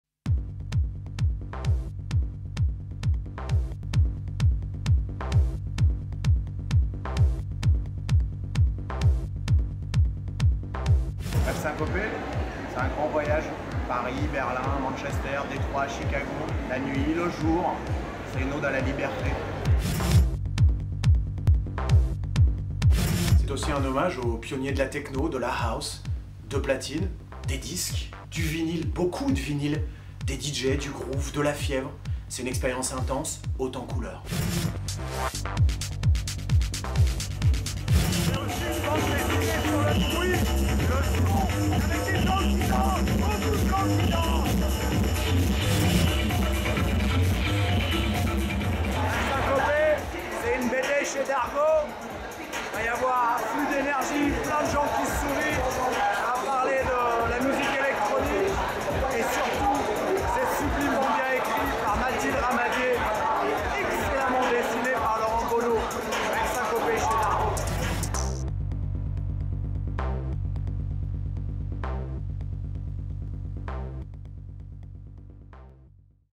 Rêves syncopés - Laurent Garnier
Rêves syncopés, la bande dessinée signée par Mathilde Ramadier et Laurent Bonneau sur la figure marquante de la musique électro, Laurent Garnier. À travers ses propres mots, un très bel hommage à la techno, au plaisir d'être seul derrière ses platines et de voir la salle en transe... De Manchester à Berlin, quelques repères biographiques suffisent, le reste est affaire de sensation, d'émotion. Bonneau a trouvé son sujet de prédilection : seuls son graphisme et ses couleurs pouvaient envoûter le lecteur comme le fait cette musique. Une symbiose impressionnante !